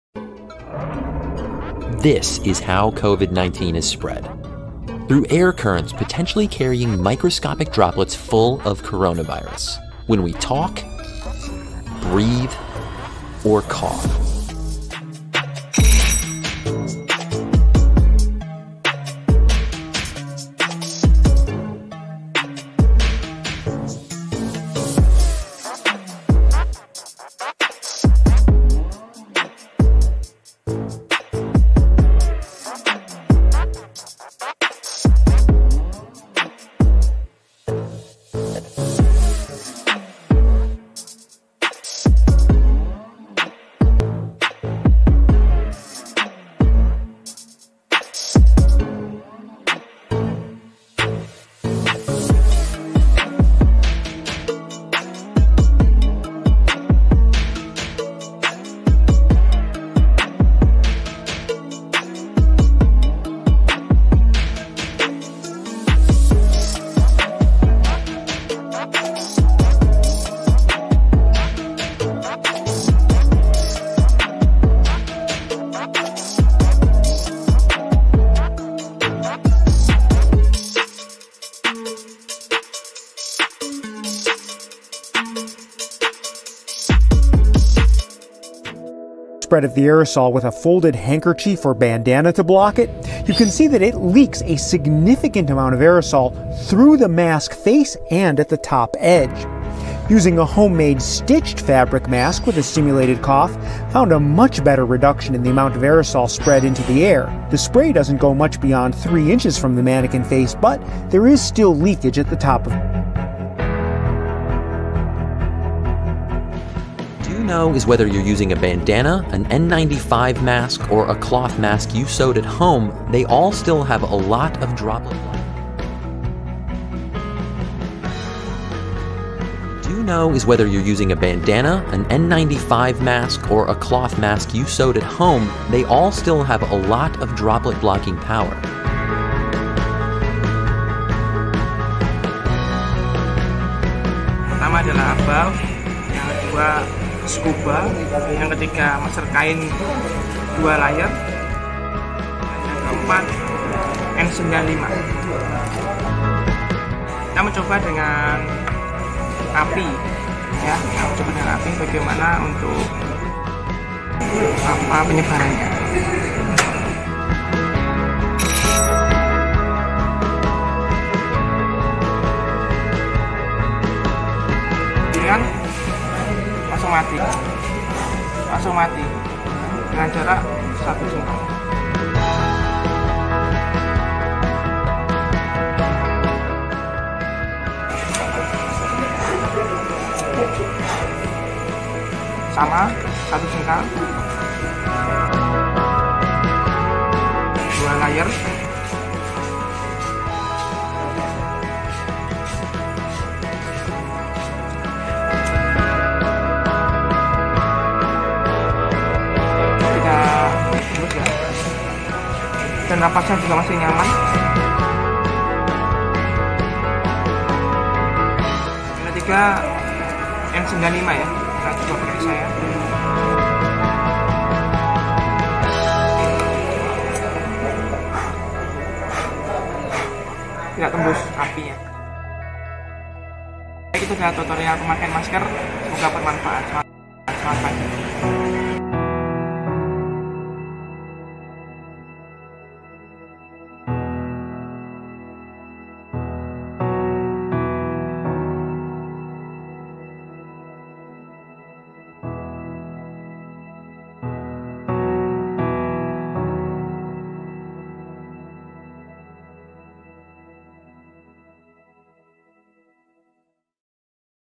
TEST MASKS ON THE MARKET vs BEST MASK ON THE MARKET
OVERVIEW = TRIAL MASK TEST IN THE MARKET - THE MOST EFFECTIVE TYPE OF MASK IN PREVENTING THE SPREAD OF CORONA VIRUS TEST MASKS ON THE MARKET vs BEST MASK ON THE MARKET

Corona virus diameter is estimated at 125 nanometers
or 0.125 micrometers, not 400-500 micrometers
where 1 micrometer = 1000 nanometers
THE MEANING OF THIS VIRUS IS VERY SMALL

This diameter was discovered by two researchers from the US, Anthony R. Fehr and Stanley Perlman
in Research publications on the US National Biotechnology Information Center Website

Another fact is the Corona Virus can last more than 10 minutes on the surface, including the hands

The World Health Organization (WHO) calls the new corona virus (COVID-19) CAN last for several hours
even a few days

Then, the Corona Virus does not die only in temperatures 26-27 degrees Celsius
THEREFORE, PUBLIC NEEDS TO GET THIS INFORMATION RELATED TO THE MASKERS THEY USE